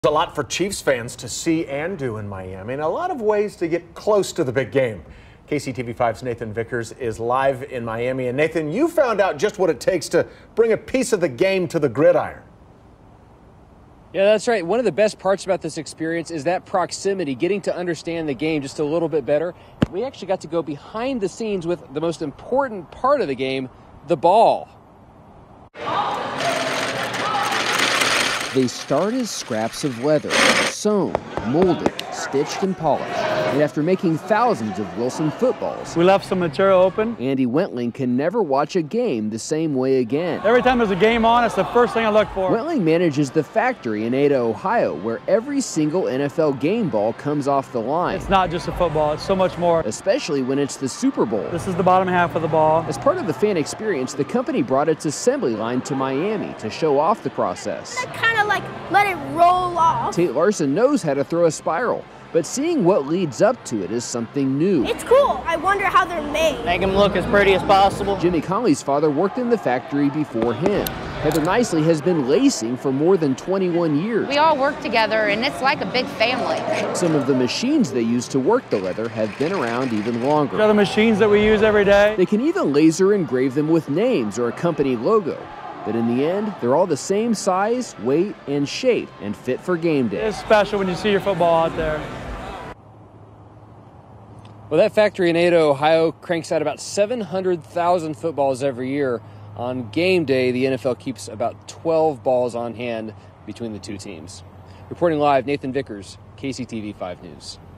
Company who makes footballs brings assembly line to Miami
Part of the experience of the Super Bowl is connecting to the game up close from the biggest players to the tiniest stitches. KCTV5 News met the people whose attention to detail goes into the most important part of the game the ball!